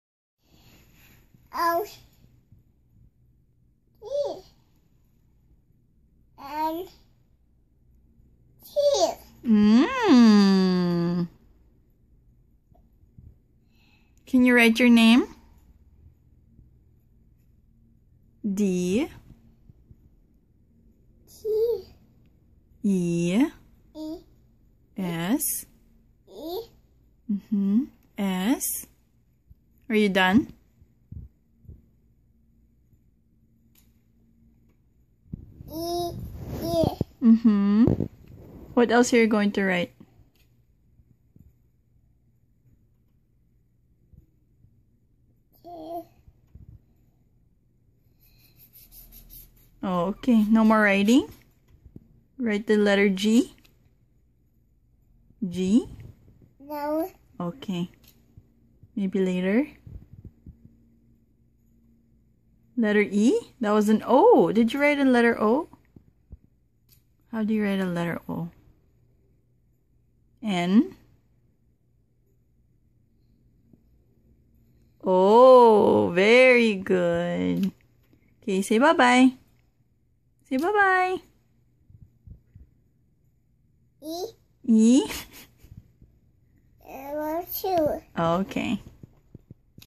Learning his letters.
This boy loves to draw and even wants to learn how to write. It's amazing how he already knows a few letters.